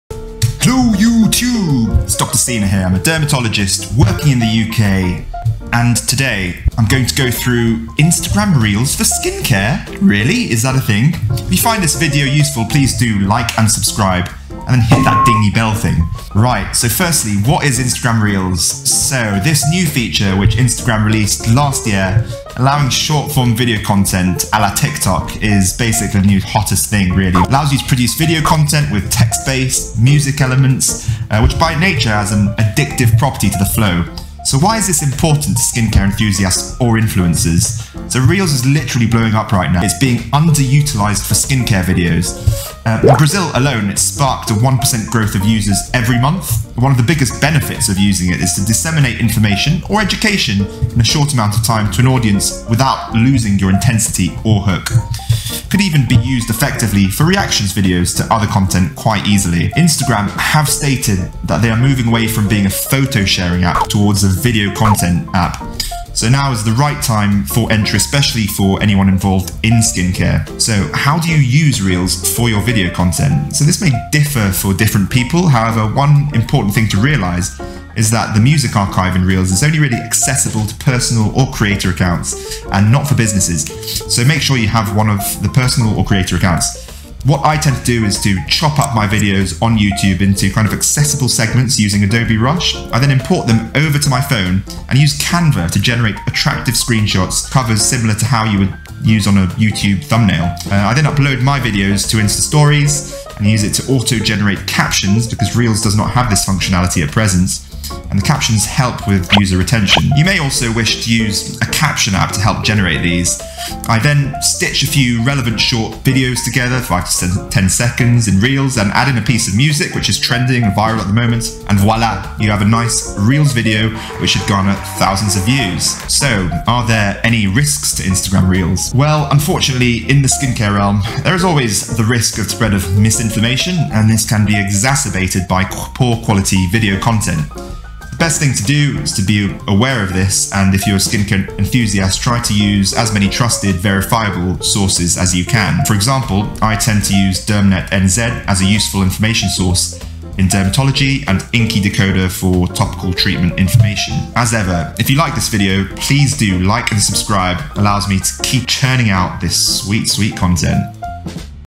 Why you NEED to have an Instagram Reels video
I go through the benefits of using Instagram Reels, especially as a skincare influencer or enthusiast. I talk about some of the features and how I go about using my youtube content to make an instagram reels video. It really still is a great time to be making great content, and doing a reels video is one of the great ways to get eyeballs on your content!